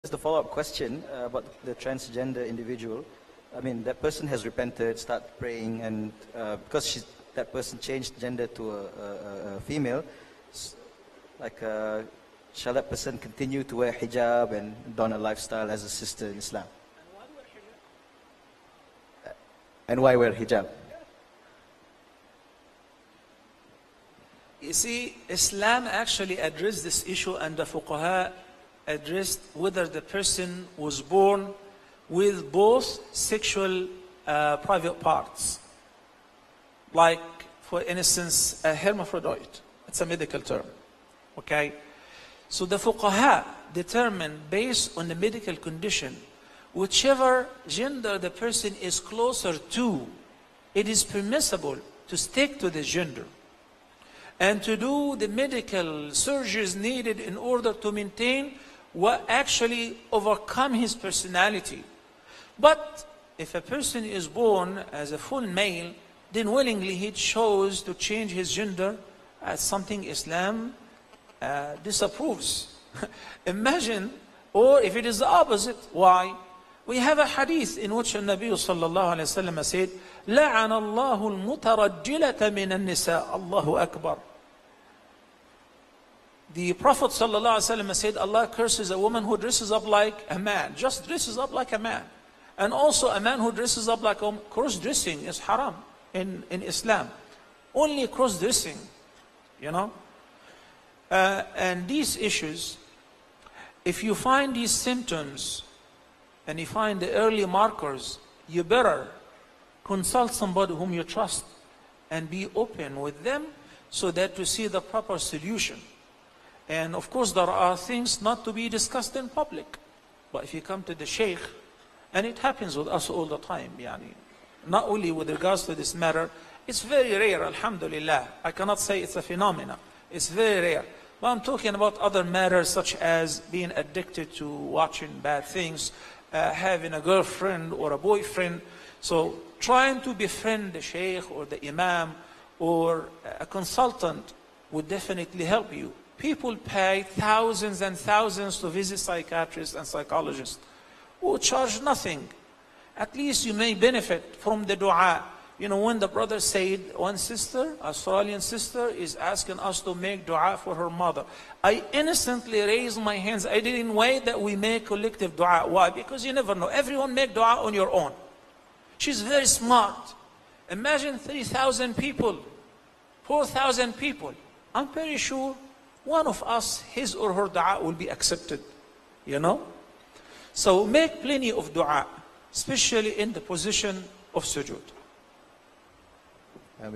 Change Of Gender From Male To Female By Muhammad Salah Q&A
Change Of Gender From Male To Female By Muhammad Salah Q&A Question and Answer